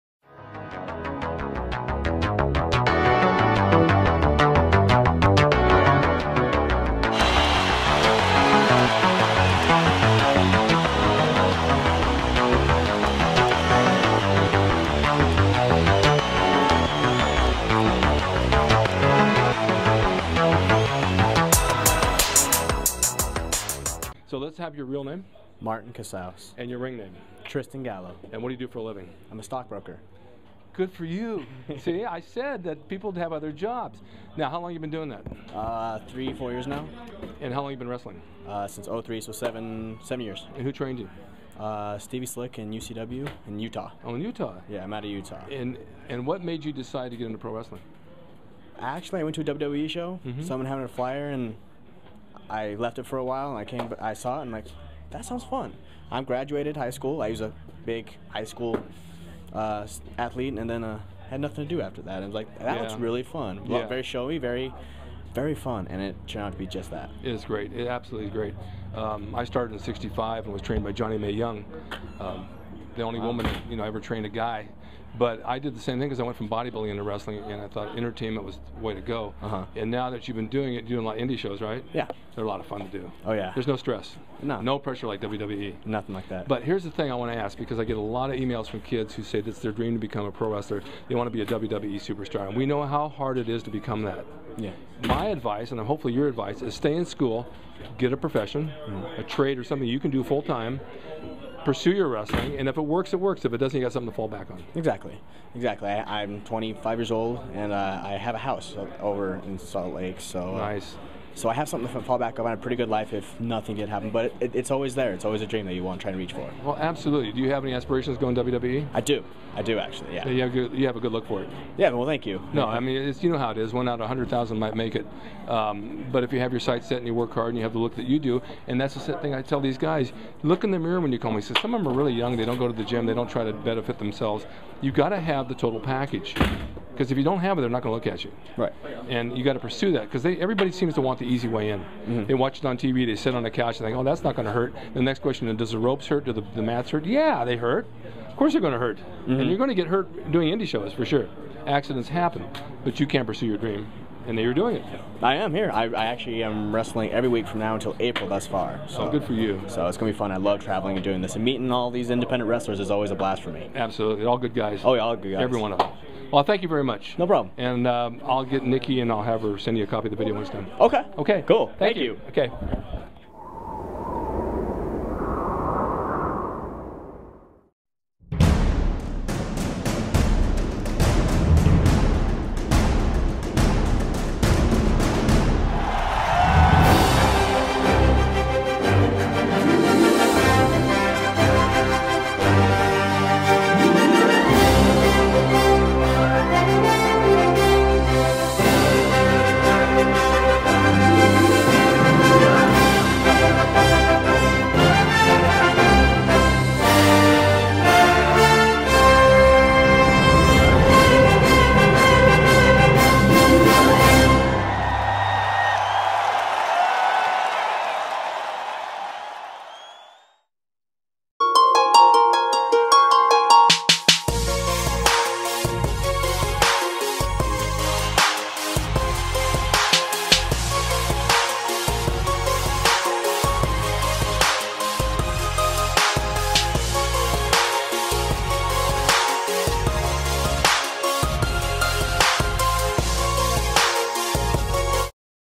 Martin from 2011 Tough Enough.mov
Ric's Corner with 'Martin', from 2011 Tough Enough Series before he went on the show. This was at an Indy show in Santa Clarita